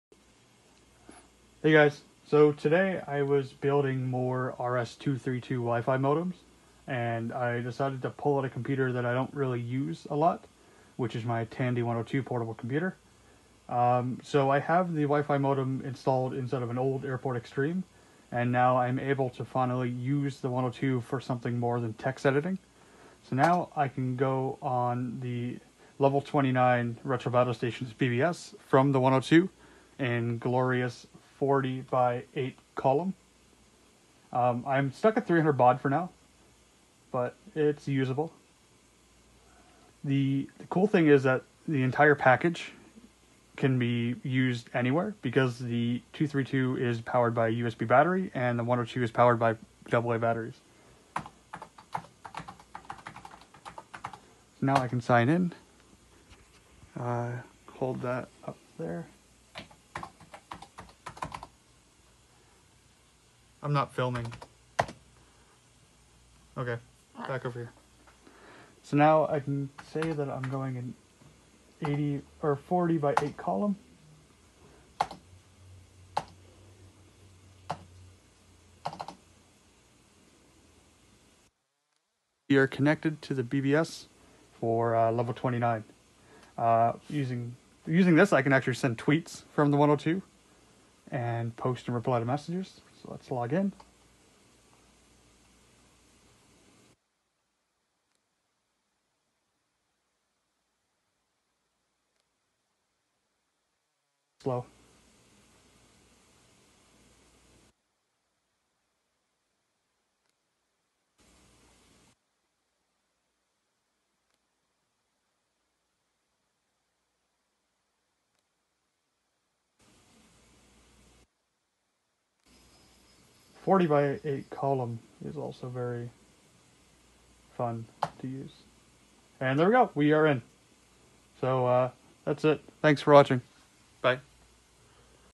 Connecting a Tandy 102 to a BBS
Connecting a Tandy 102 to a BBS using a wifi modem